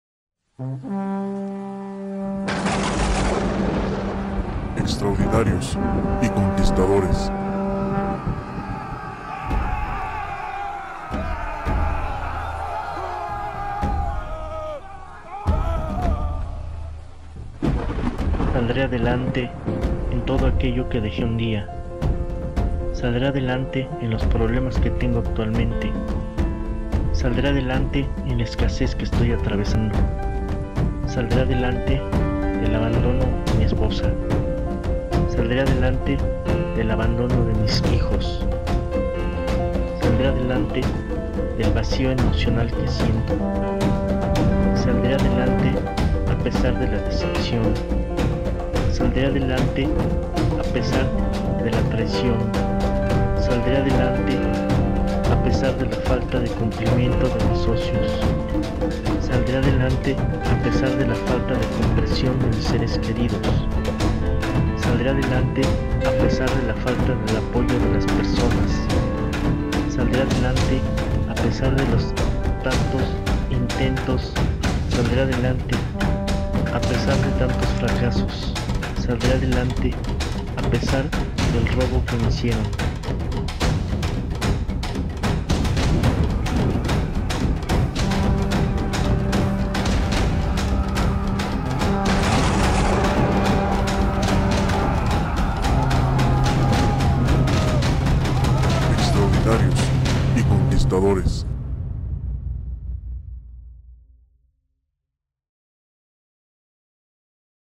¿Cómo salir adelante? ¿Cómo salir del abandono? ¿Cómo dejar el fracaso? (TODO ES POSIBLE)
¿Debemos salir adelante?
¿Como salir adelante?
¿Donde salir adelante?
¿Cuando salir adelante?
¿Рodemos salir adelante?

A pesar de tantos fracasos, problemas, conflictos, contra tiempos, enfermedad, abandono, escasez, falta de apoyo, falta de comprensión, falta de recursos; podemos salir adelante.

El primer paso para salir adelante es creer que podemos hacerlo.

Saldré adelante en todo aquello que deje un día.

Saldré adelante en los problemas que tengo actualmente.
Saldré adelante en la escasez que estoy atravesando.
Saldré adelante del abandono de mi esposa.
Saldré adelante en abandono de mis hijos.
Saldré adelante del vacío emocional que siento.
Saldré adelante a pesar de la decepción.
Saldré adelante a pesar de  la traición.
Saldré adelante a pesar de la falta de cumplimiento de mis socios.
Saldré adelante a pesar de la falta de comprensión de mis seres queridos.
Saldré adelante a pesar de a pesar de la falta de apoyo de las personas.
Saldré adelante a pesar de los tantos intentos.
Saldré adelante a pesar de tantos fracasos, a pesar del robo que me hicieron.